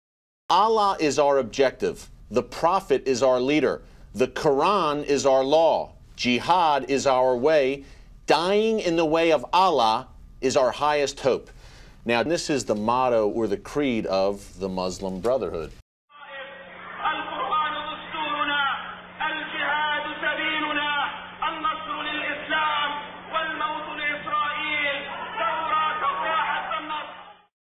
Sami Al-Arian Pledges Muslim Brotherhood Motto
Sami Al-Arain you pleaded guilty to terrorism charges and was a former professor at the University of South Florida is caught pledging the Muslim Brotherhood back in 1991.